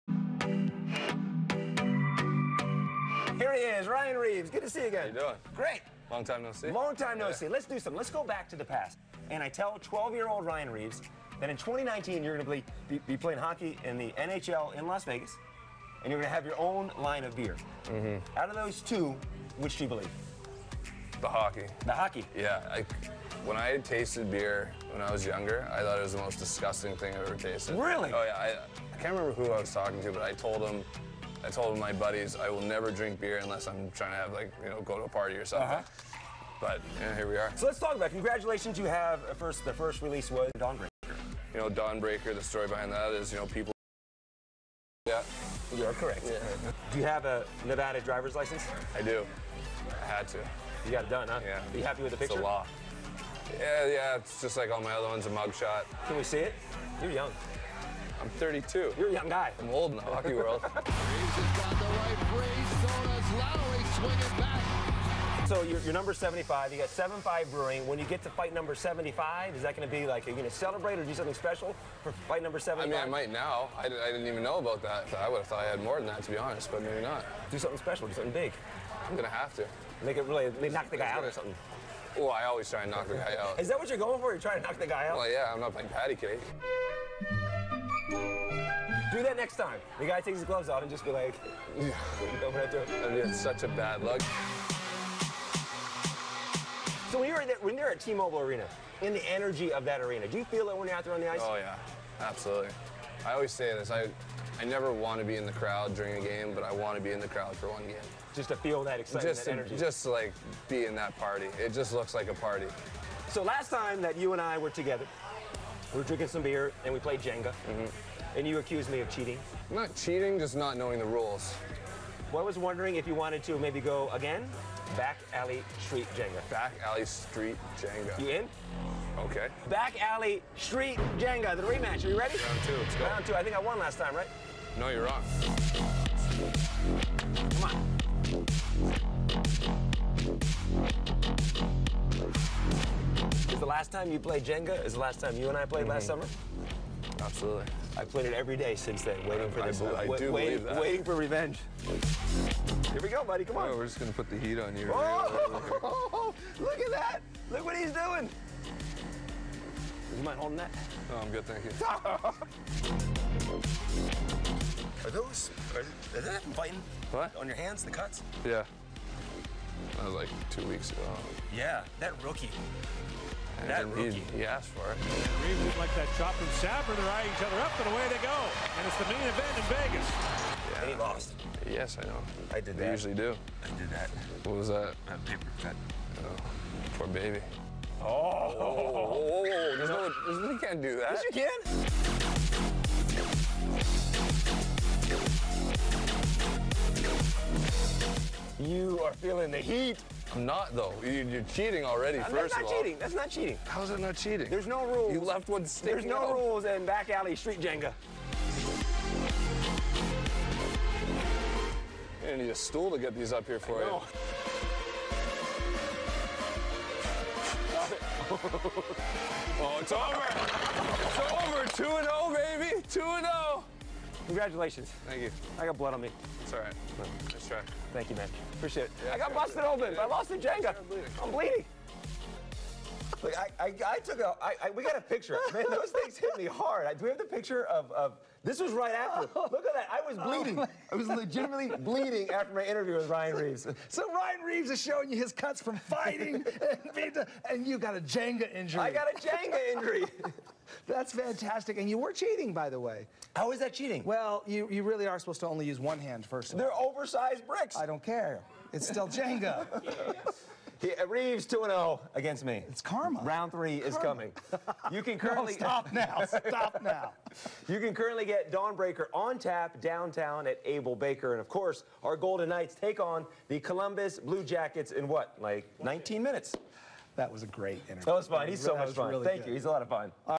Ryan Reaves Talks Dawn Breaker And Plays Jenga
Ryan Reaves sat down with Las Vegas NOW on 8 News Now to talk about his new line of beer called Dawn Breaker. Plus we played a little back alley street Jenga.